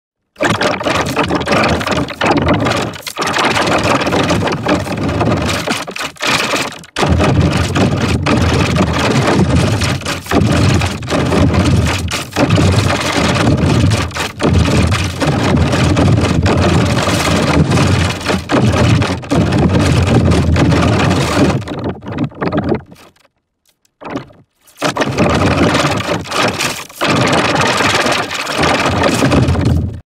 Grubhub Ad - Delivery Dance but it's in 3.0 DUH
My First Grubhub Ad Meme video.

Effect by TimelyCentralVision1530.
Original (pls, don't watch but ad)